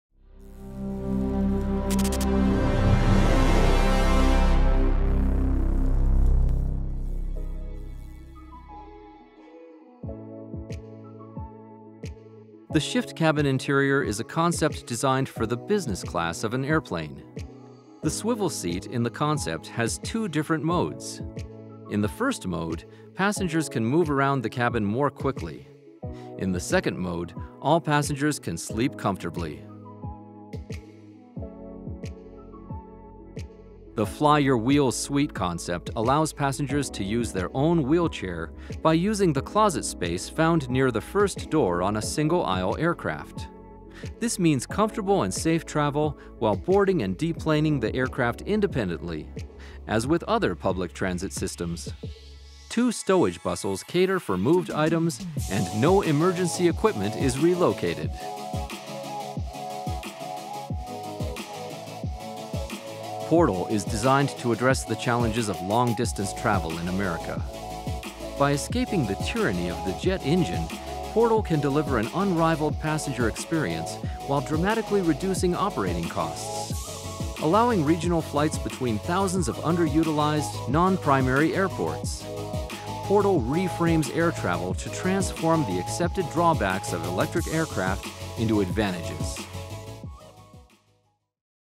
Crystal Cabin Awards 2022 Finalists: University
The University category is a playing field for imaginative concepts with a clean-sheet approach to a range of issues and situations. Carlos Gatti, student at Wichita State University, has come up with a novel way of accommodating wheelchair passengers. The Fly Your Wheels Suite replaces the front left closet on aircraft like the Boeing 737-800 with a multi-purpose module that can function as a safe parking berth for a wheelchair user during a flight. Rather than taking away existing seating, the idea would let airlines add one paying passenger – the wheelchair user. Jiayi Yu from the University of Reutlingen designed the Shift Cabin Interior, a study that allows operators to configure seats in multiple positions for work or relaxation, both along the axis of the seat as well as to the side. Aside from the advantage of flexibility, the concept lets airlines fit more seats into the same business class space. Ken Kirtland of the Georgia Institute of Technology has high aims with Portal: to create an electric short-haul airline based around an innovative aircraft that emphasises a calmer flight experience at slower speeds and fantastic views.